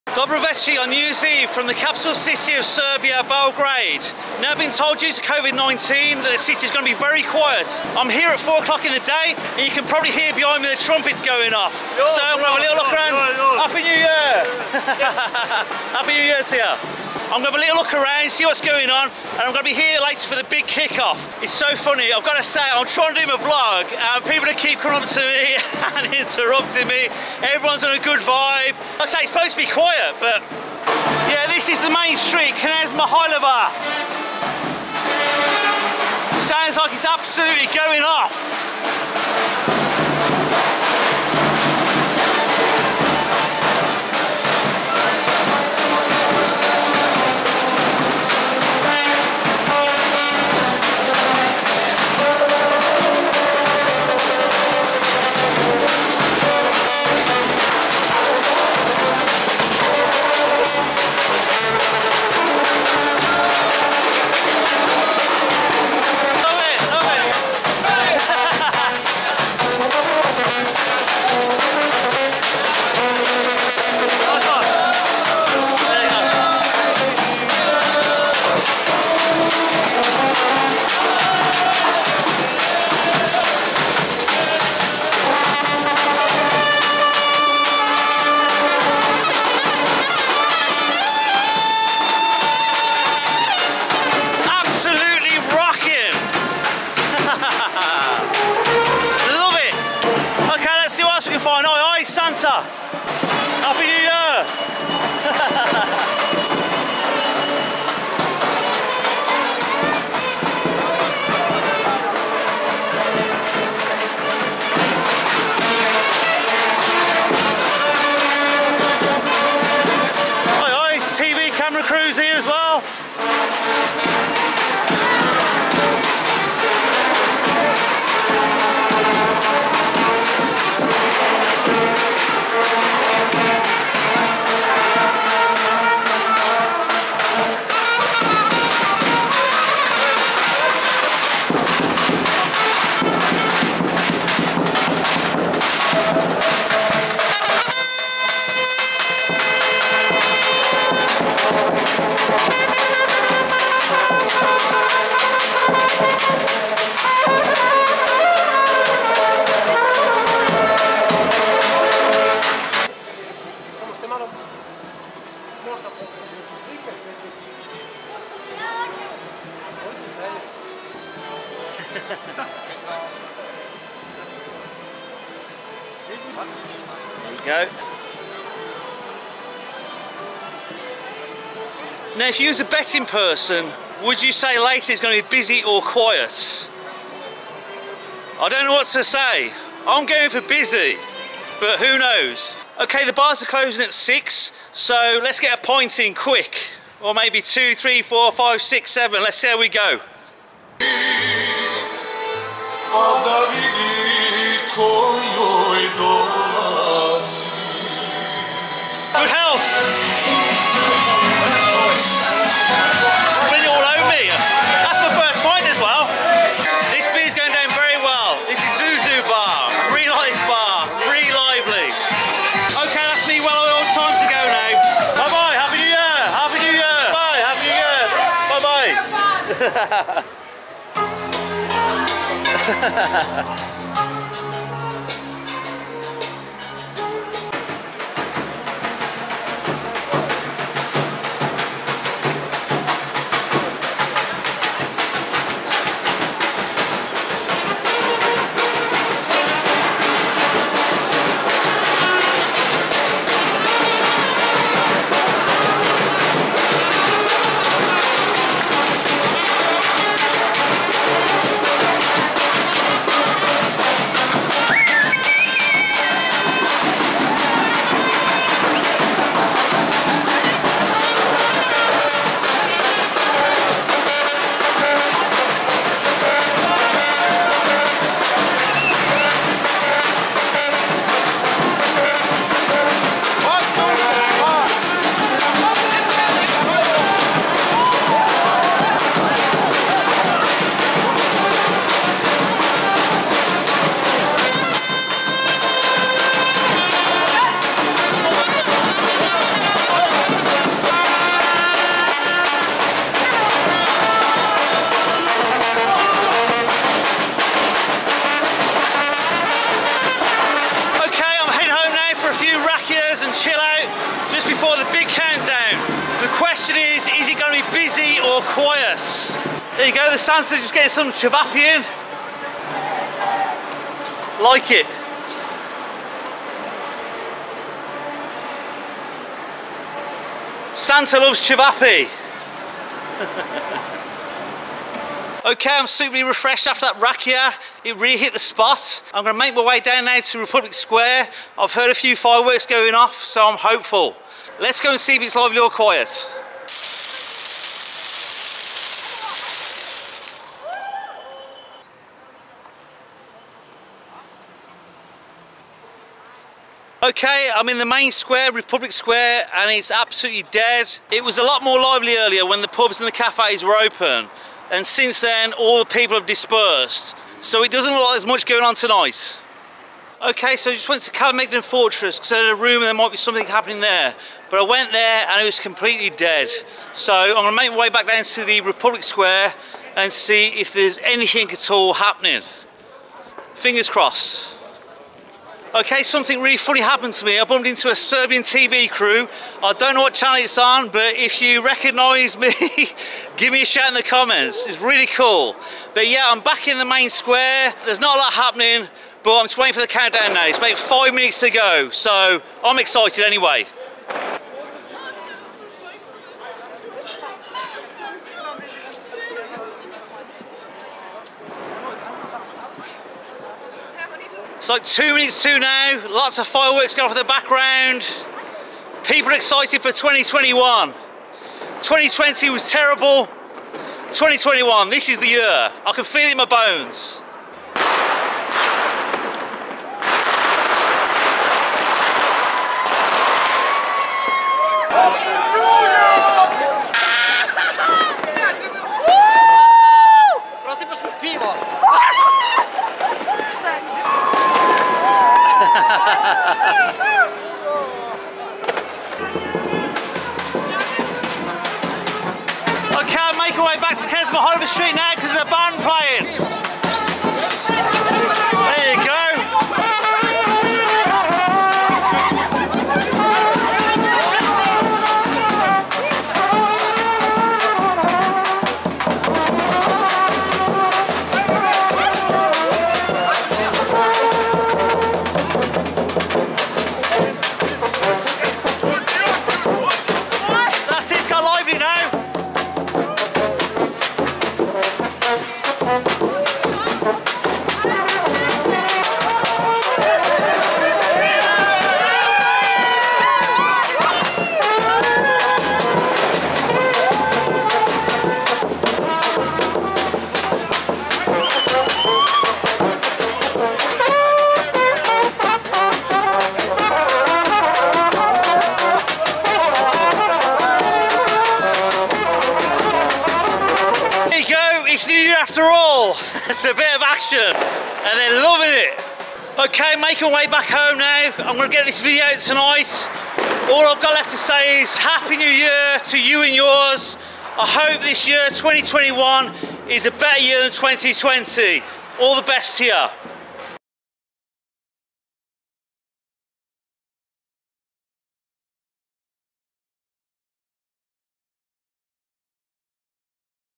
A Covid New Years Eve In BELGRADE SERBIA
It's my first time in Belgrade Serbia, also my first time experiencing a Covid New Years Eve!
Join me on a walk through/tour of the capital city Belgrade to check out what New year's Eve 2021 in Serbia looks like.
There will be a good chance I will be like many others nursing a hangover New Years day but a bit of Serbian Burek should sort that right out!
I wish you and your family a Happy New Year!

Shout me on Instagram @ birmze